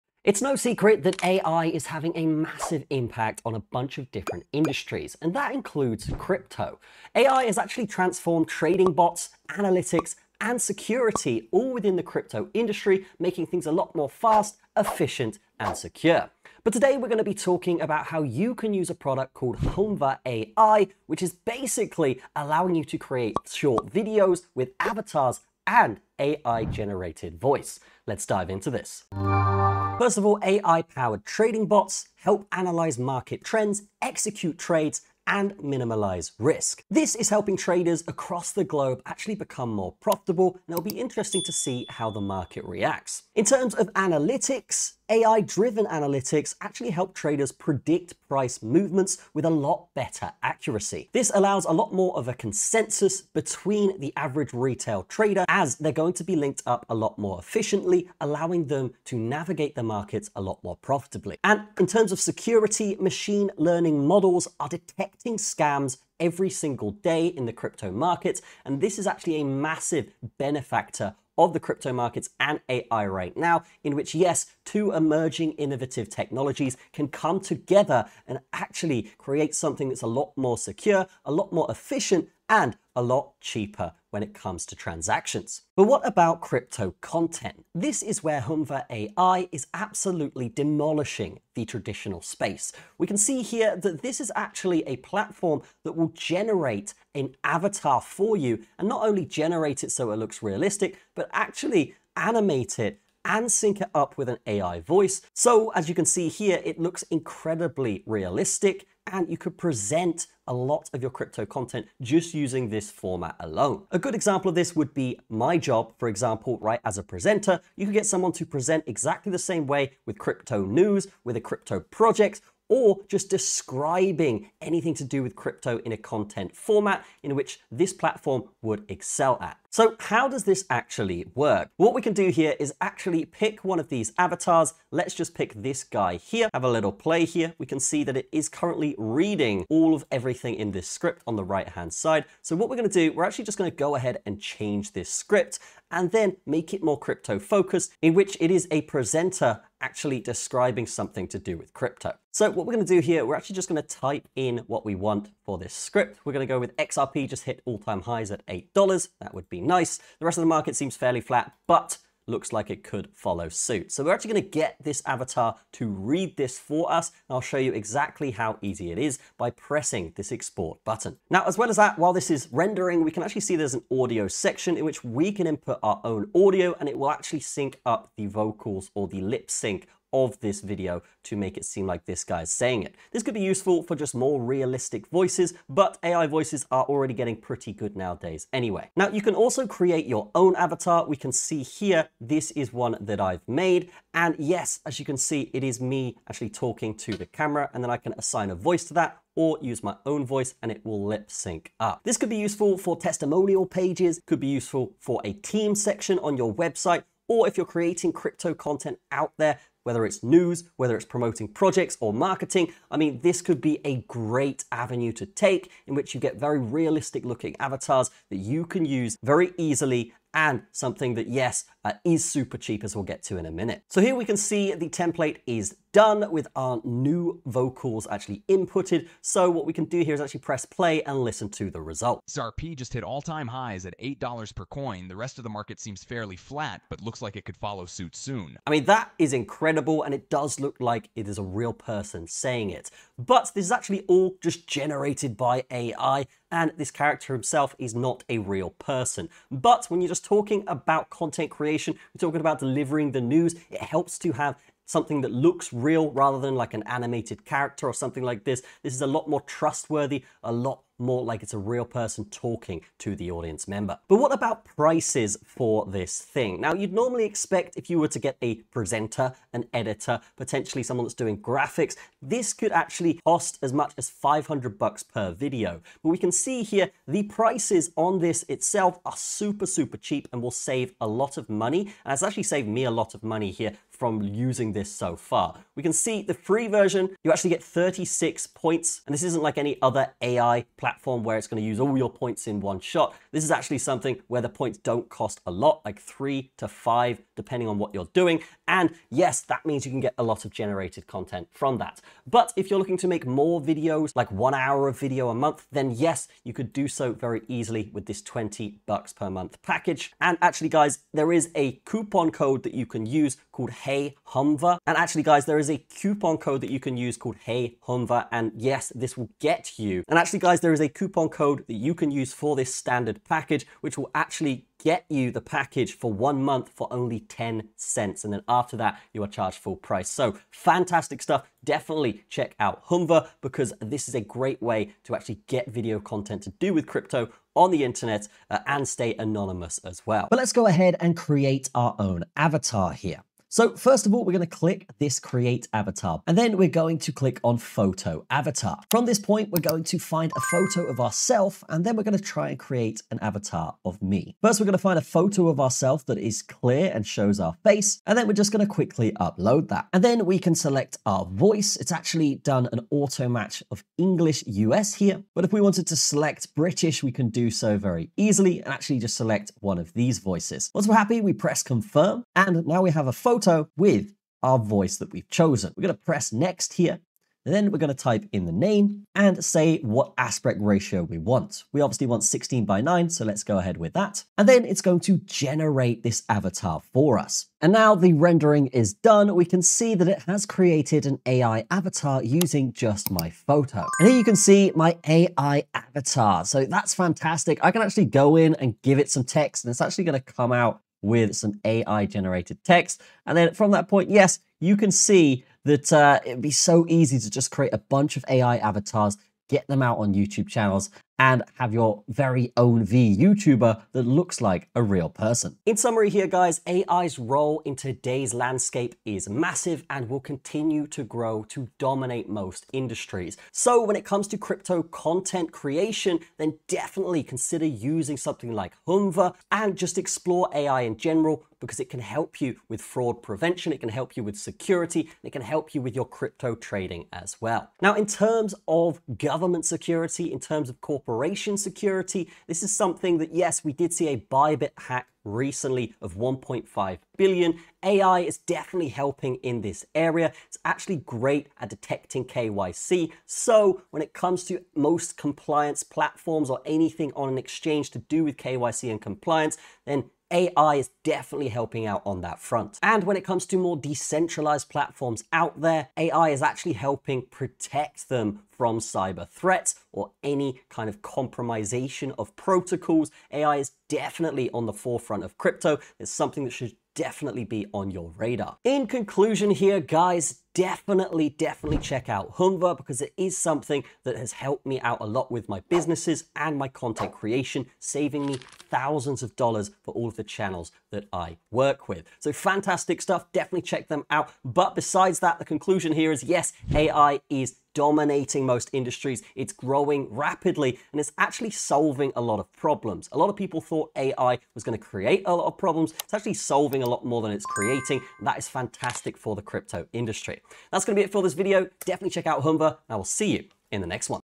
AI Crypto Is Changing Everything! Meet - Humva
Ai Crypto | Artificial Intelligence Crypto | Best Crypto to Invest in 2025

🎯 Get Your Videos Ranked FAST! 🚀 Dominate YouTube with YTRanker!

Discover how Ai Crypto is revolutionizing trading, security, and content creation in the blockchain space! 🚀 From AI-powered trading bots to Huma AI's game-changing avatar technology, this video breaks it all down for you in easy-to-follow steps.

🔥 What You’ll Learn:
- How AI trading bots analyze market trends, execute trades, and minimize risks for better profitability.
- The role of AI-driven analytics in predicting price movements and boosting your trading strategies.
- How machine learning is transforming crypto security by detecting scams and preventing hacks.
- A deep dive into Huma AI's realistic avatars and voice-syncing tech — perfect for crypto content creators looking to stay anonymous or enhance engagement.
- How Huma AI cuts costs for creating professional content, saving you thousands of dollars!

💡 Why It’s a Game-Changer:
- Save time and money with automated solutions for trading and content creation.
- Stay ahead of scammers with AI-enhanced security tools that protect your assets.
- Create engaging crypto content in minutes without hiring expensive presenters or editors.

📌 Ready to take your crypto game to the next level?

CHAPTERS:
00:00 - AI in Crypto
00:30 - AI Trading Bots
01:25 - Humber AI
06:57 - AI in Crypto Security
07:54 - Conclusion